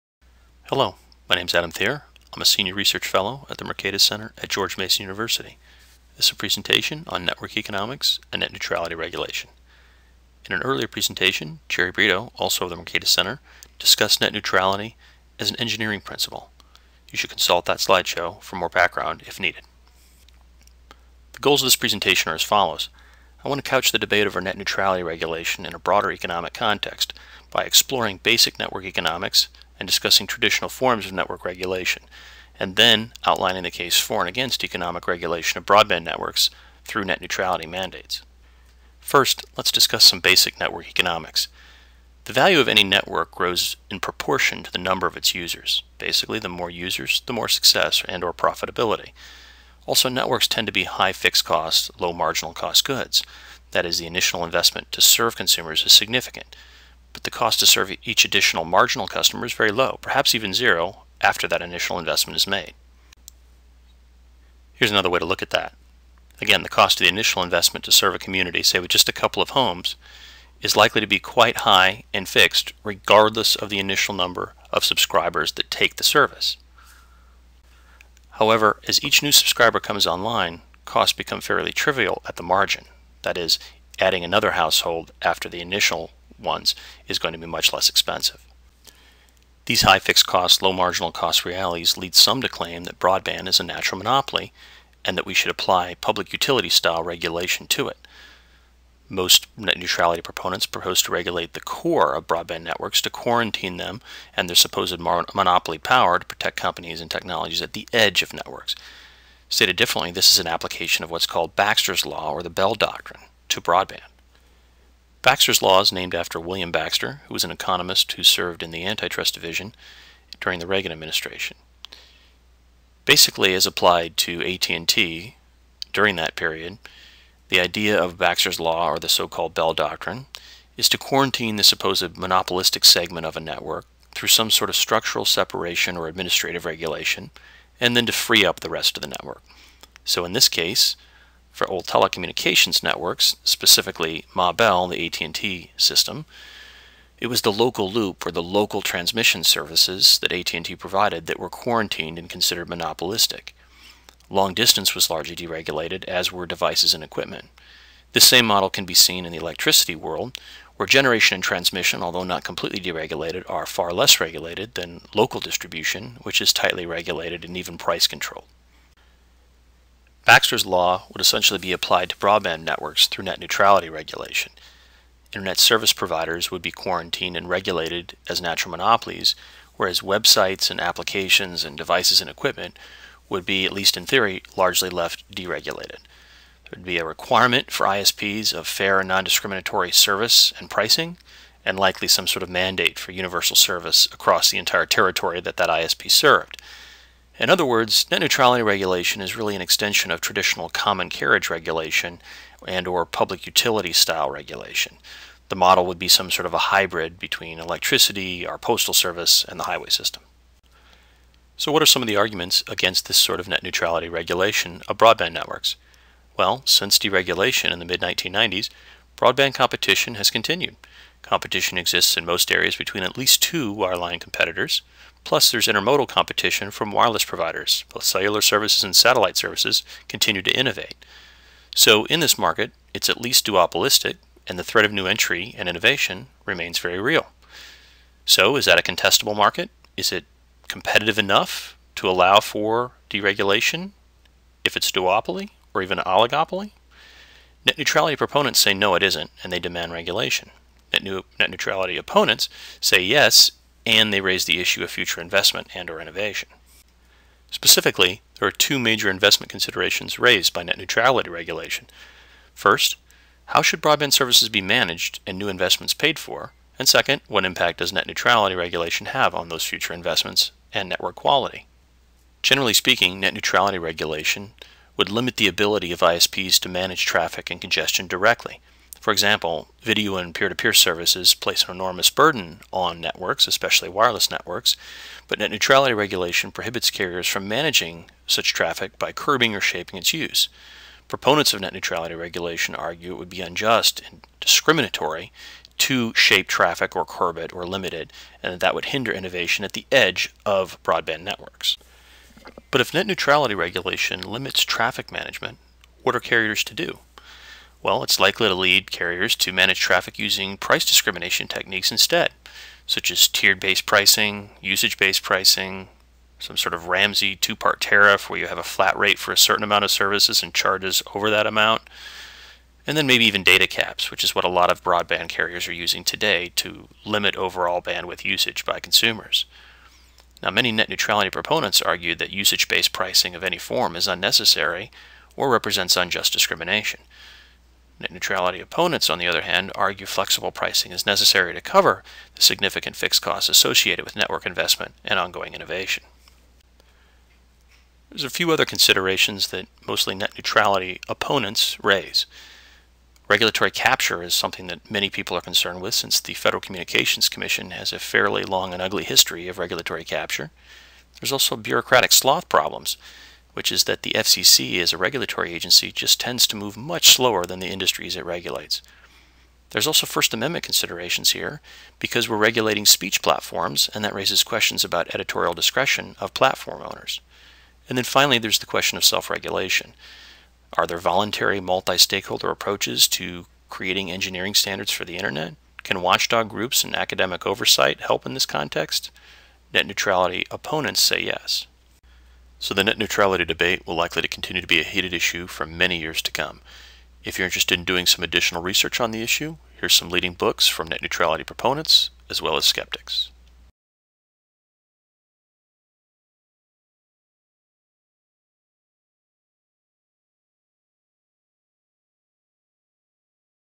Network economics and 'net neutrality' regulation by Adam Thierer
What are the pros and cons of legally mandated net neutrality?